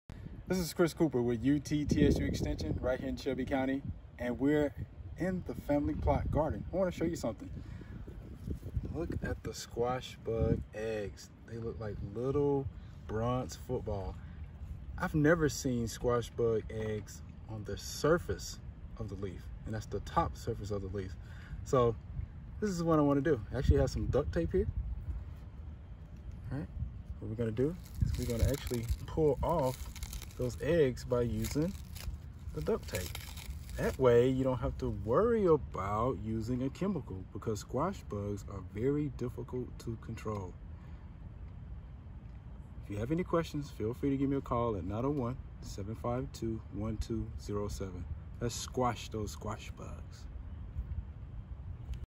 Squash Bug Eggs by UT Extension Agent Chris Cooper
UT Extension Agent Chris Cooper shows a unique method to control squash bugs.